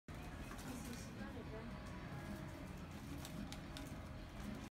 NECA Chucky Child's Play SOLAR moving figure/Body Knocker - KAYY'S Collection
No batteries required, activated by light. Available at KAYY'S Collection at Place Vertu Shopping Centre.